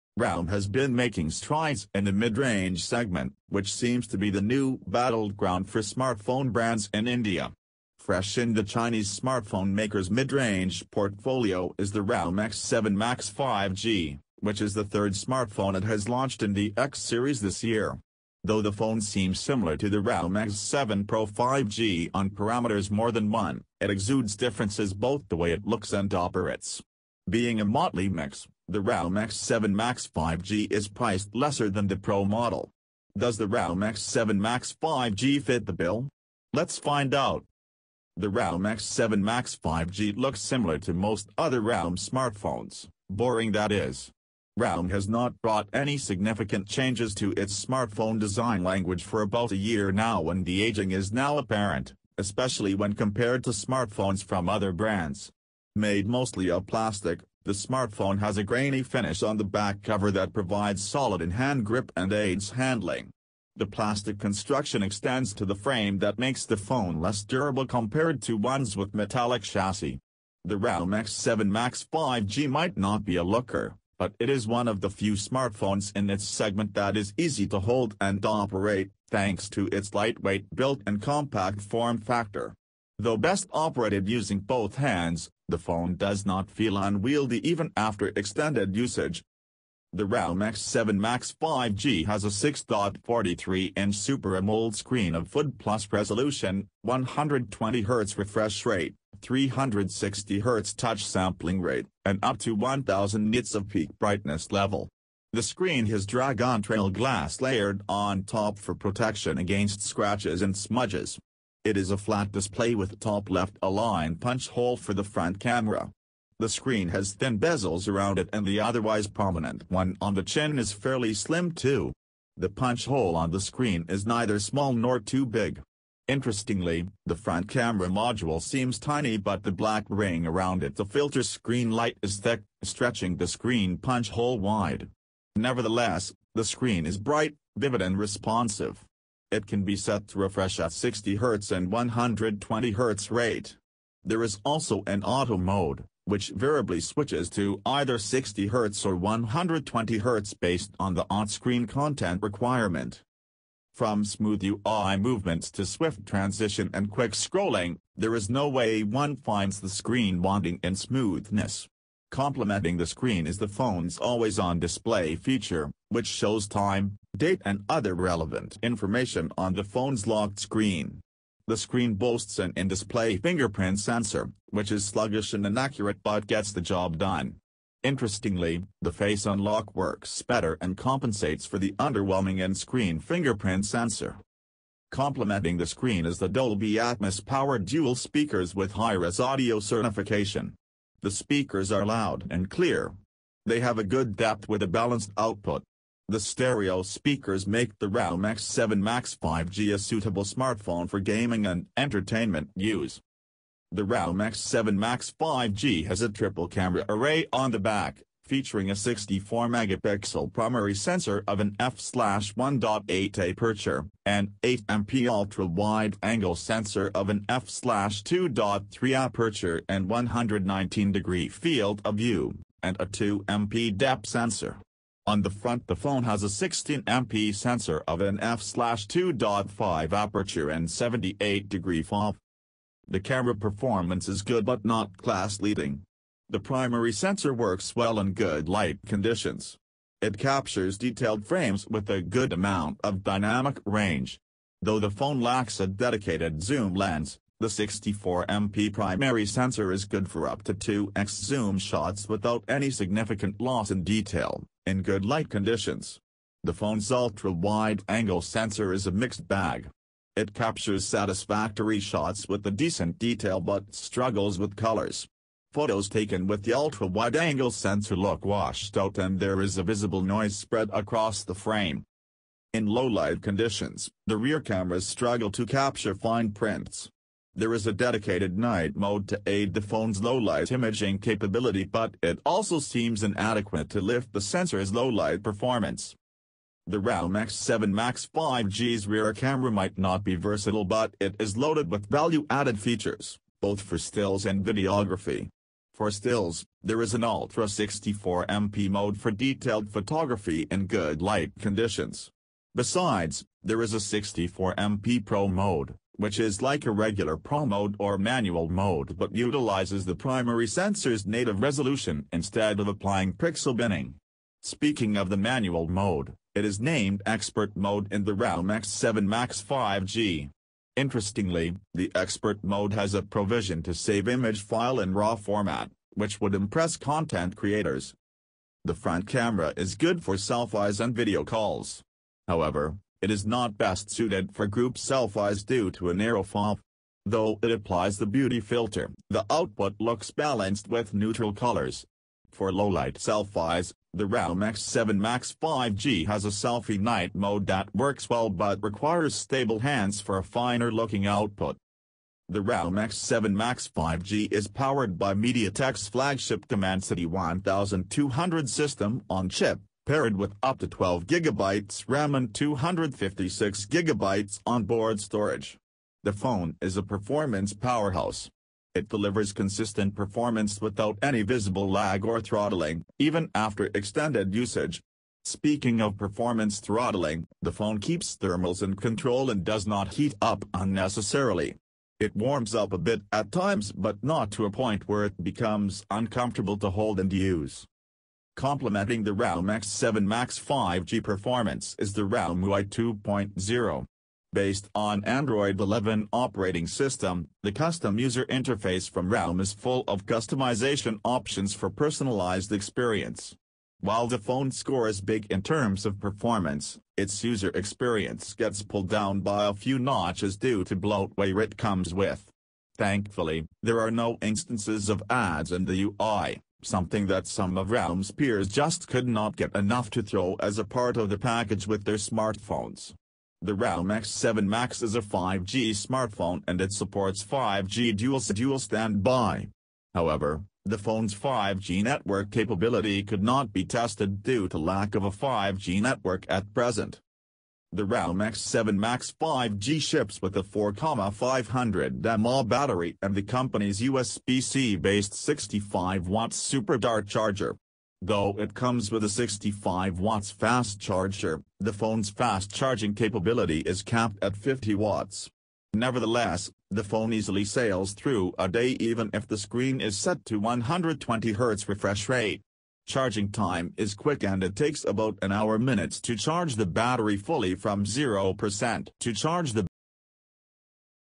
Realme X7 Max 5G review: Solid package with a bit of something for everyone
SOCIAL